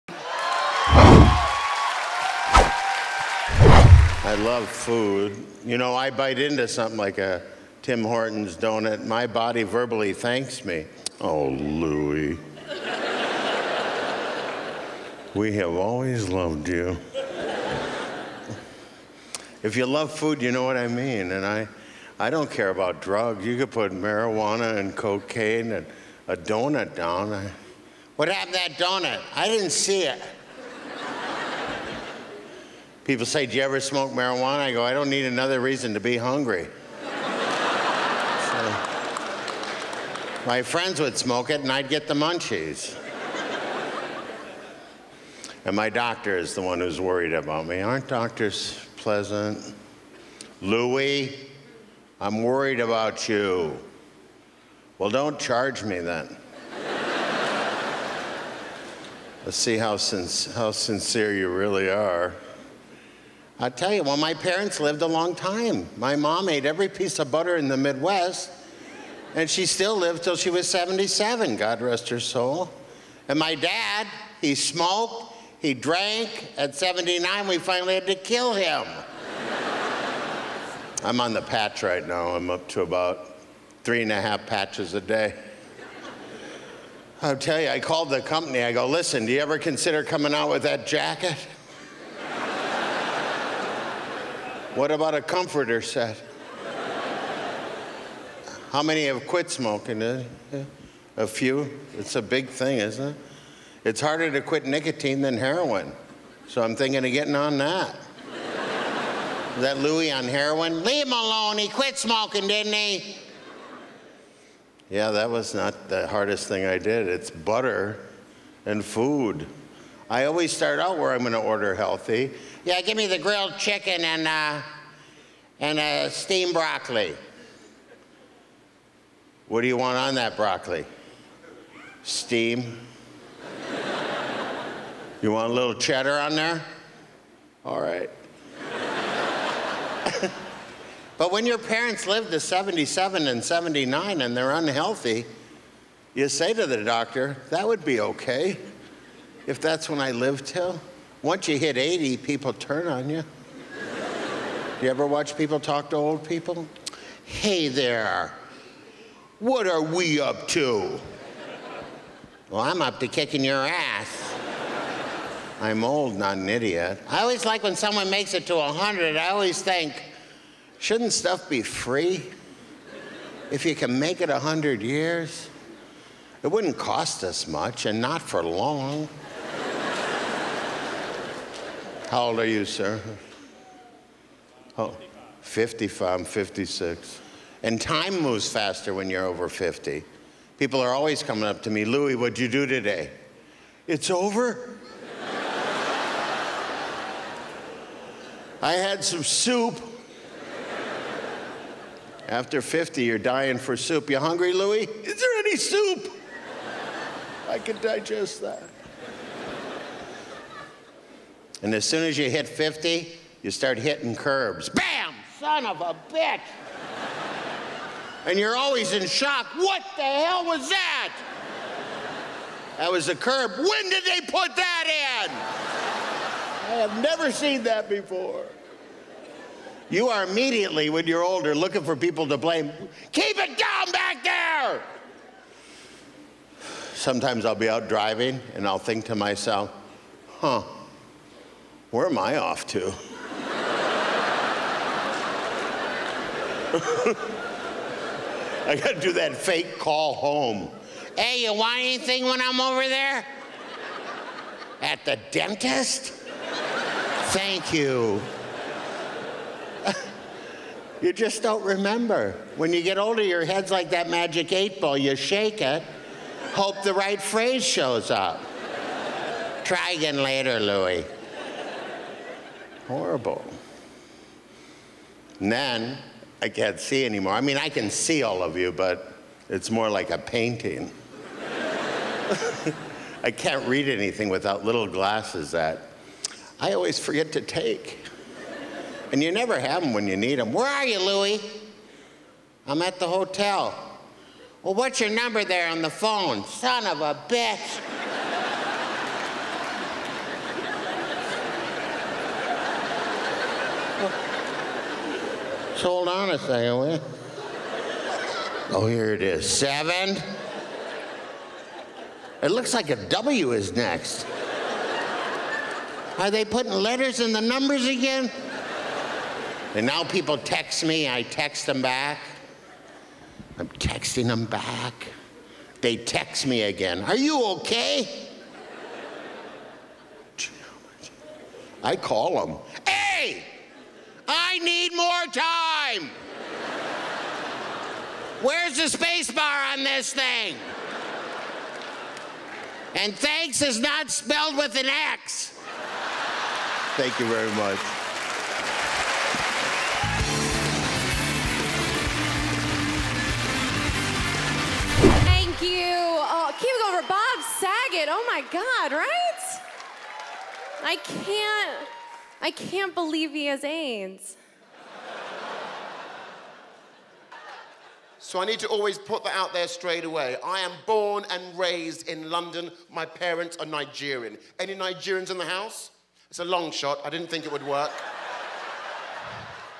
Louie Anderson - Everything Changes When You're Over 50
LouieAnderson, comedian, actor, and television host, best known for his cartoon series "Life with Louie", hosting "Family Feud" and for his role as Christine Baskets on FX series "Baskets", brought his nimble comedy to the JustForLaughs Festival in 2009.